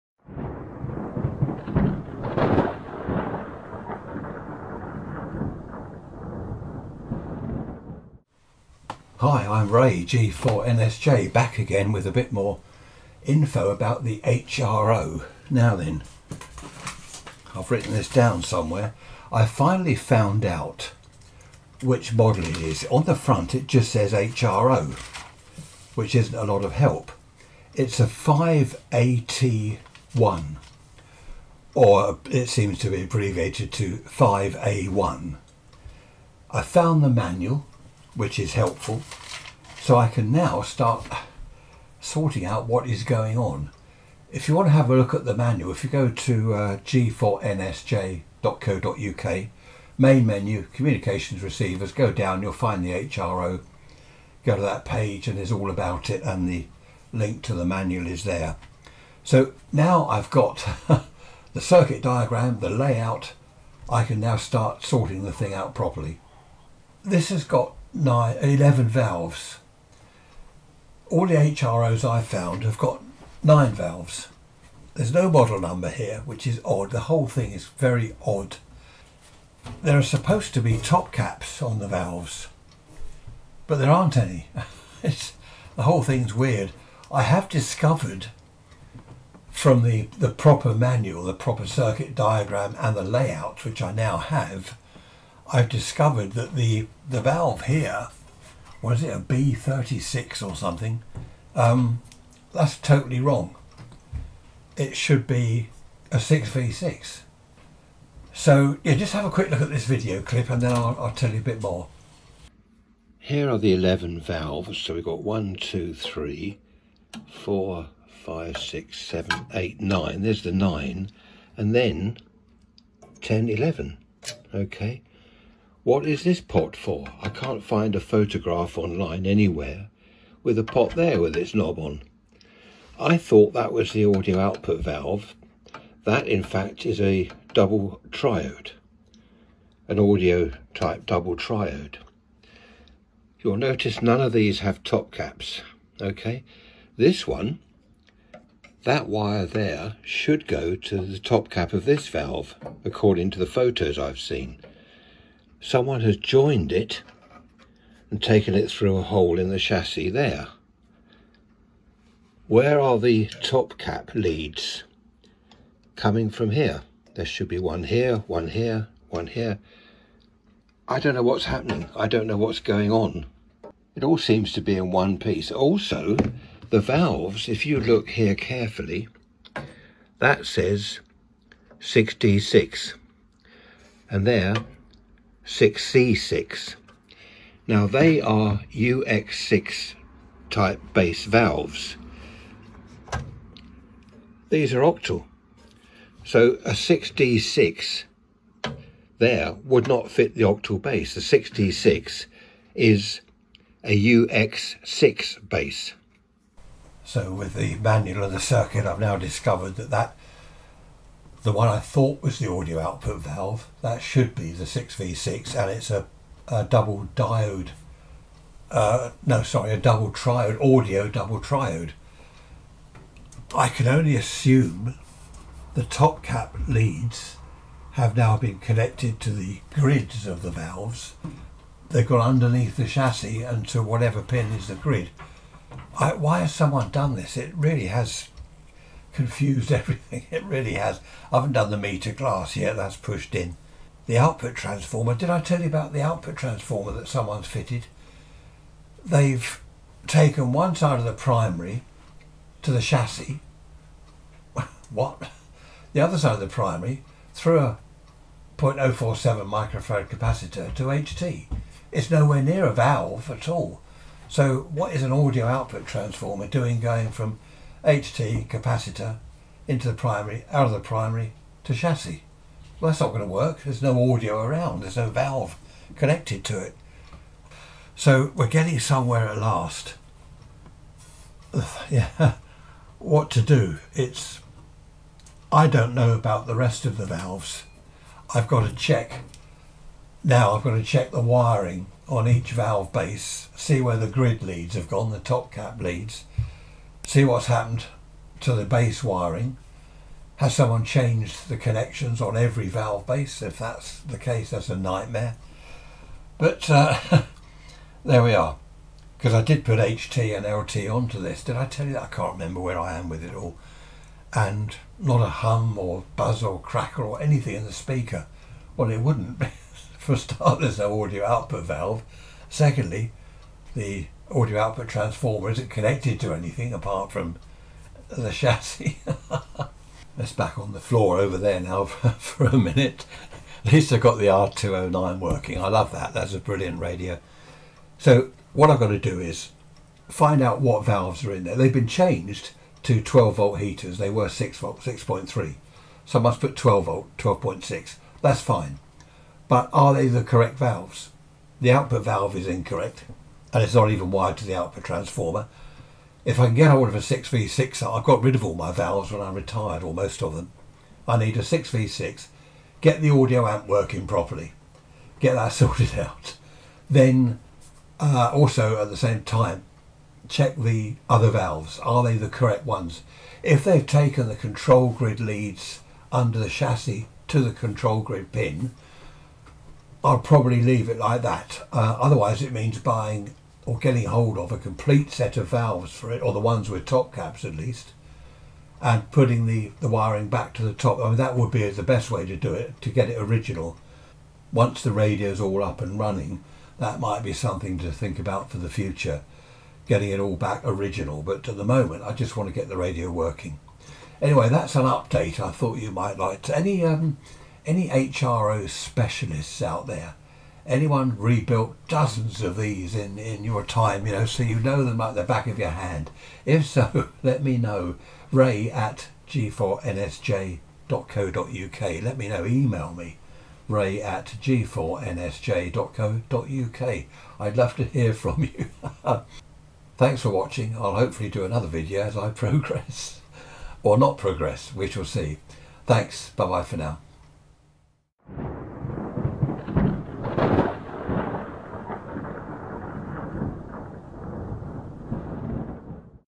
G4NSJ - National HRO 5TA1 - 5A1 communications receiver
An update on the HRO receiver problems. I've now discovered the model number of the HRO. It's 5TA1 or 5A1. I also have the correct manual. Strange audio output transformer connections, valves with no top caps, no audio output valve... This radio has problems!